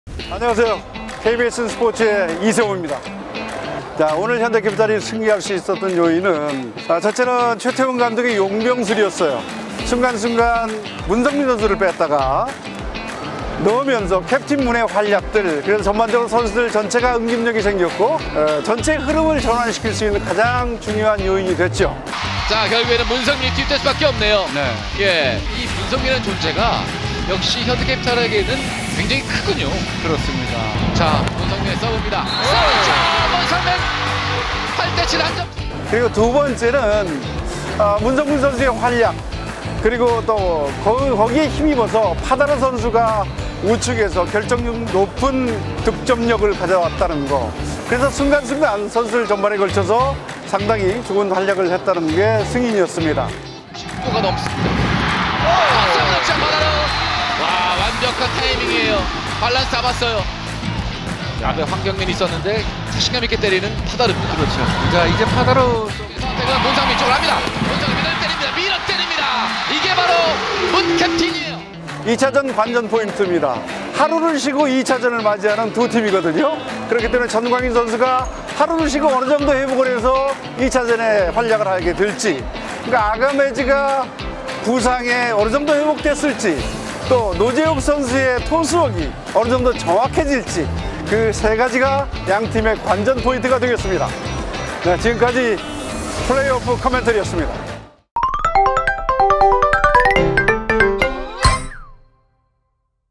[PO코멘터리] KBSN SPORTS 이세호 해설위원의 남자 1차전 분석!
KBSN SPORTS 이세호 해설위원이 분석한
남자부 플레이오프 1차전!